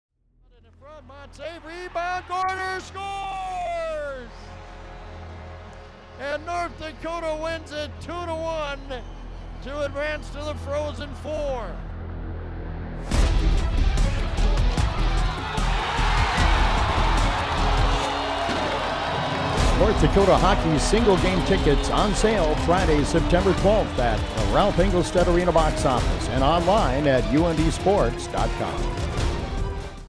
UND Hockey single-game tickets on sale Friday, Sept 12
Both UND Men's and Women's Hockey single-game tickets go on sale Friday, September 12th. Get you tickets at the Ralph Engelstad Box Office or online at UNDSports.com